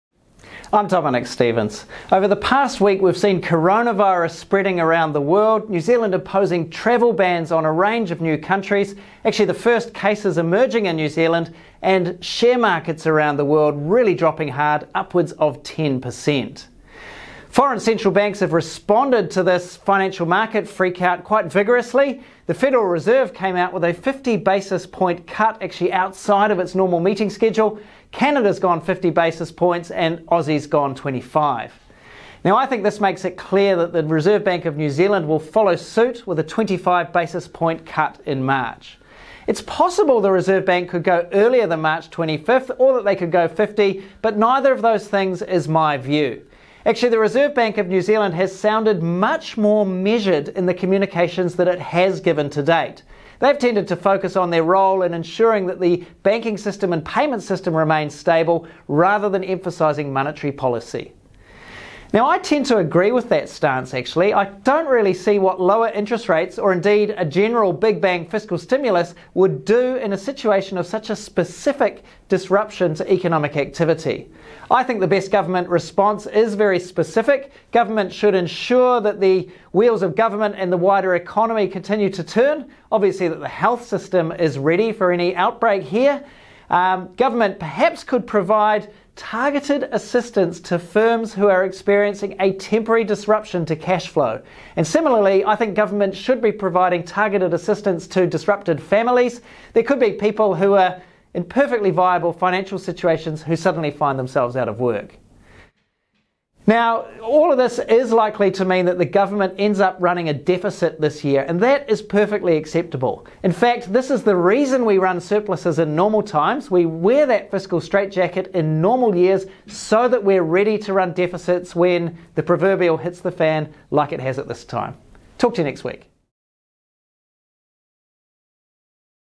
A specific solution for a specific problem
Global financial markets have swooned in response to Covid-19. Central banks overseas have rushed to reduce interest rates, but the Reserve Bank of New Zealand is likely to respond more moderately. I think that’s the correct course. This is a very specific disruption to economic activity that warrants a very specific government response.